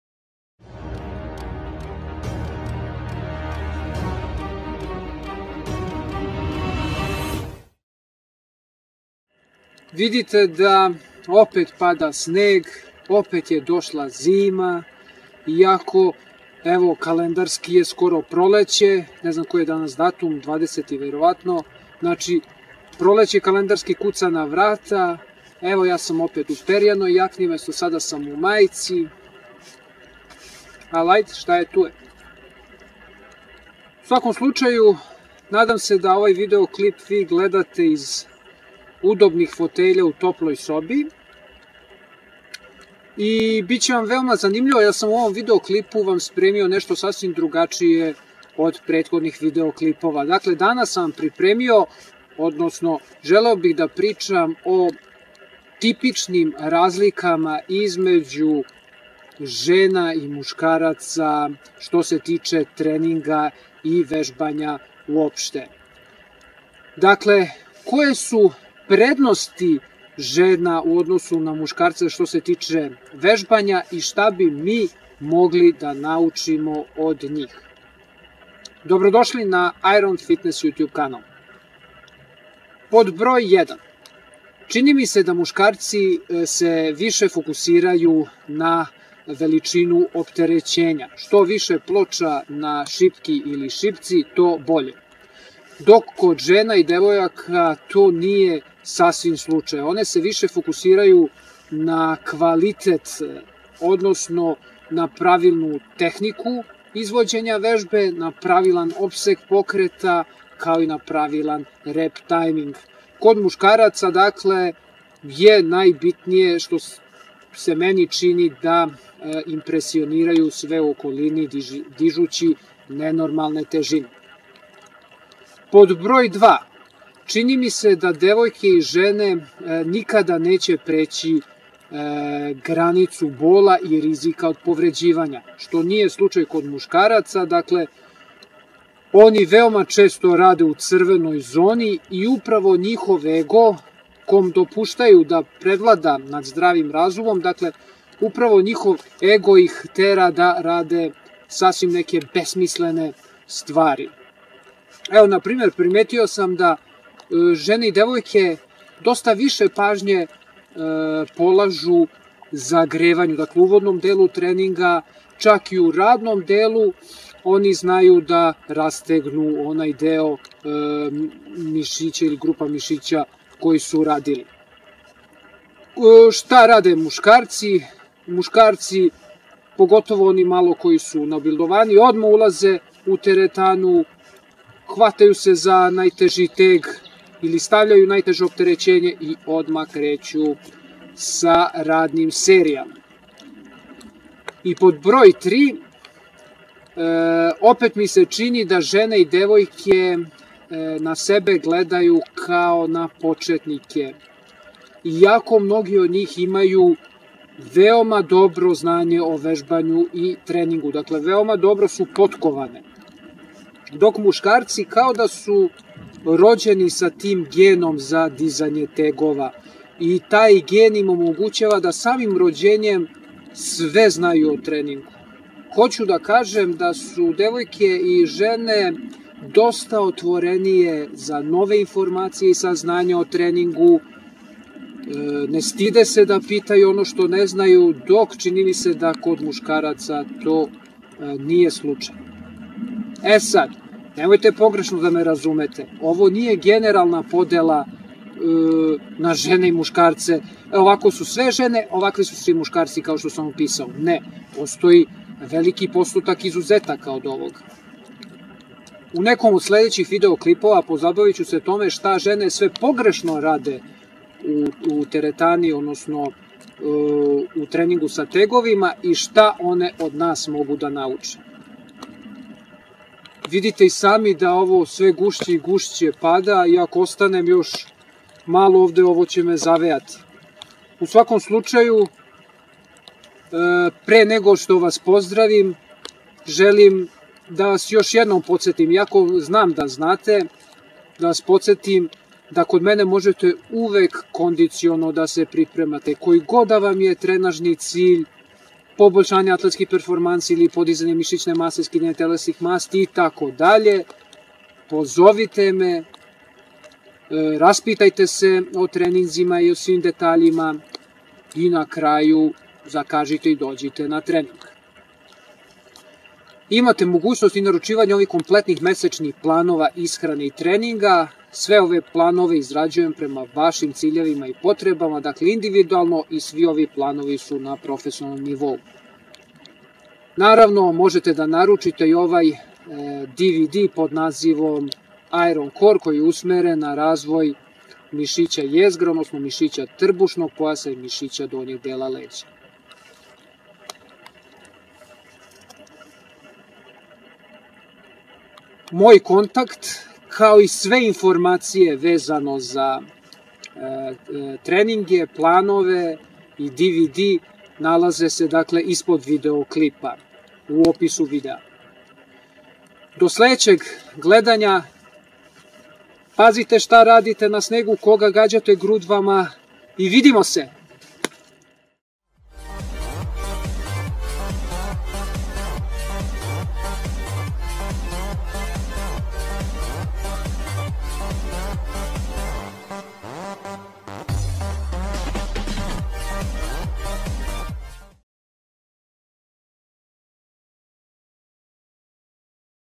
ŠTA NAS ŽENE MOGU NAUČITI O TRENINGU
Koje su razlike između žena i muškaraca što se tiče treninga u teretani...
Slobodno me možete kontaktirati na:

Na sledećim linkovima se možete detaljnije informisati o trenažnom dvd-u, planovima ishrane i treninga i treninzima:

Autor video klipa: Kondicioni trener Igor Protić